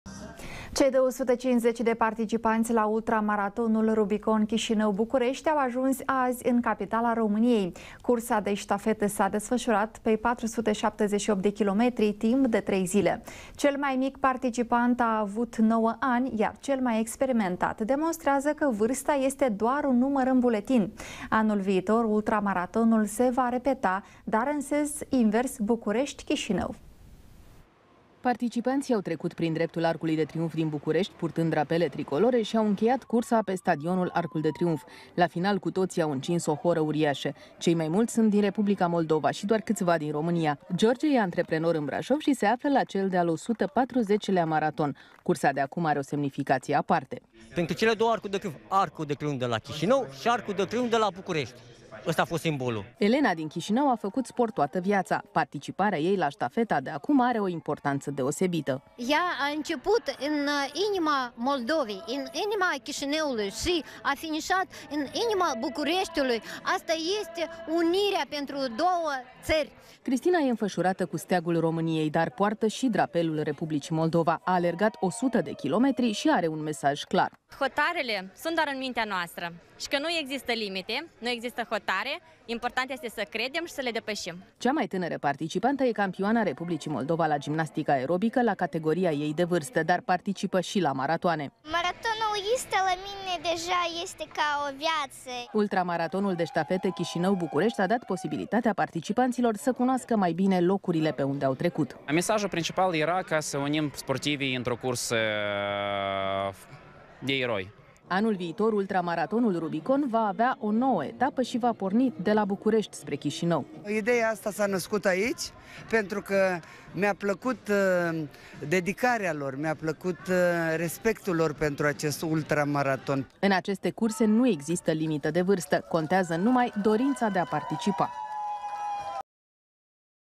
Ultramaratonul Rubicon Chișinău-București s-a încheiat. Participanţii au ajuns în capitala României
Cei 250 de participanți la Ultramaratonul Rubicon Chisinau-Bucuresti au ajuns azi in capitala Romaniei. Cursa de stafeta s-a desfasurat pe 478 de kilometri, timp de trei zile. Cel mai mic participant a avut 9 ani, iar cel mai experimentat demonstreaza ca varsta este doar un numar in buletin. Anul viitor, ultramaratonul se va repeta, dar in sens invers, Bucuresti-Chisinau.